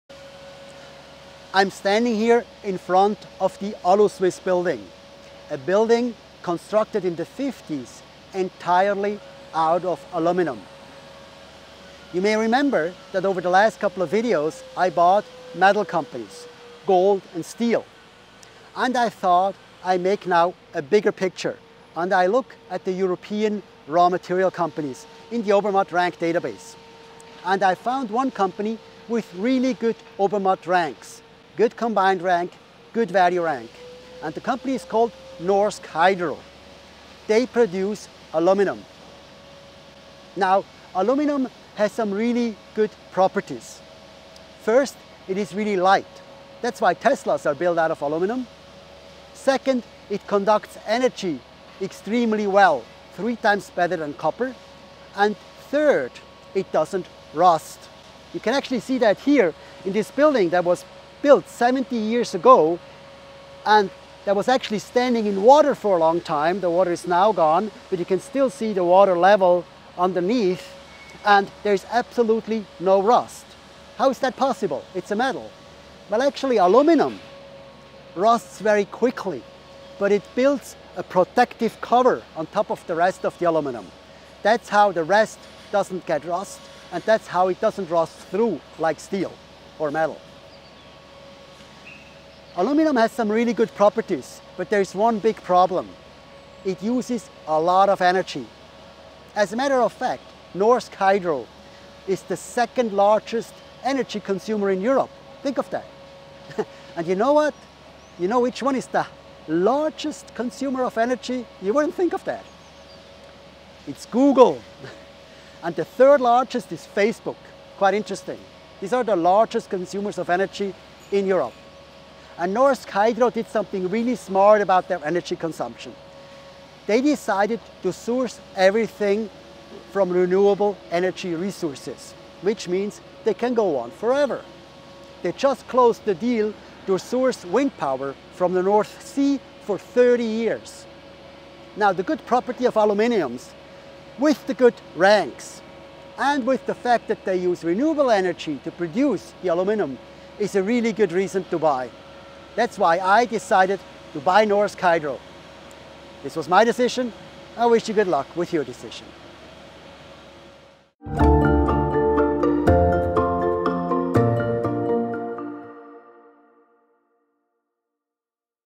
Norsk Hydro Aluminium - strong metal and stock
The European steel industry has hit rock bottom. That makes its stocks cheap – and me, in the fashion of Warren Buffet – curious. That’s why I checked the Obermatt rankings for European raw material stocks. The result? The Norsk Hydro stock has excellent rankings.
The company from Norway produces aluminium, a material that has many applications. Teslas for instance are made completely from aluminium, just like most electric cars. It’s lighter and harder than steel (and for this reason, as some Tesla drivers may know, more expensive to repair).
However, aluminium is also more expensive because producing it requires huge amounts of energy. Norsk Hydro is the second-largest energy consumer in Europe. That’s why the company has secured wind energy from the North Sea for the next 30 years. All of the energy Norsk Hydro uses comes from renewable energy sources.
I am convinced – if only for the reason that the value rank is 88 and that the company shows excellent growth and secure financing. I’m buying 1,000 shares of Norsk Hydro. Because I believe in aluminium, electromobility and wind energy.

Easy investing.
Obermatt Stocks Update and the Free Handbook helps you.